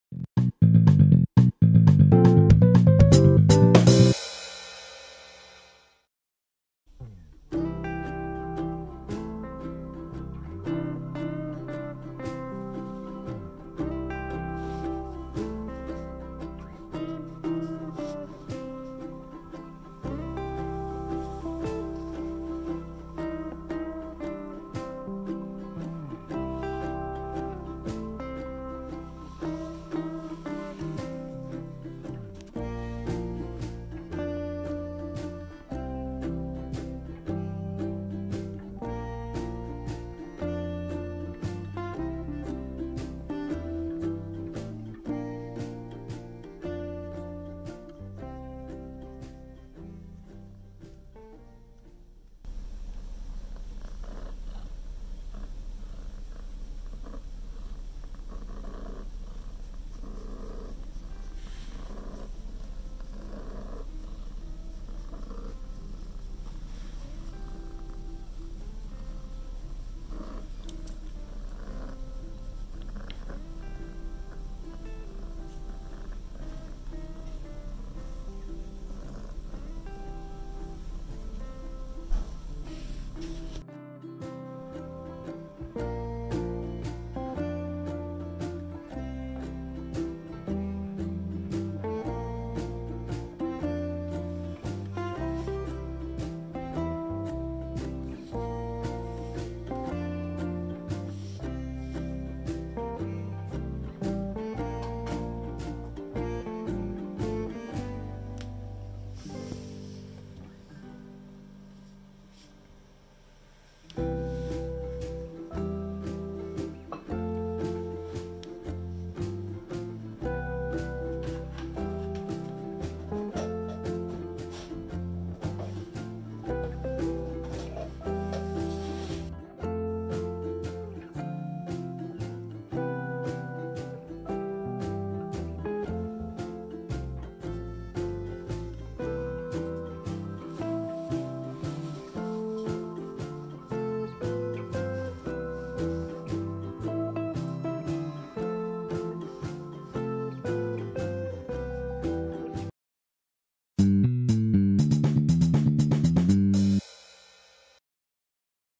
なでたいときとなでられたいときが一致しないかもしれない猫🙀/My cat doesn't always want to be petted when I want to pet🐈
猫が何かを訴えたくて近寄ってくることがありますが、結局何なのかわからないこともありますよね？😸ねこだから😽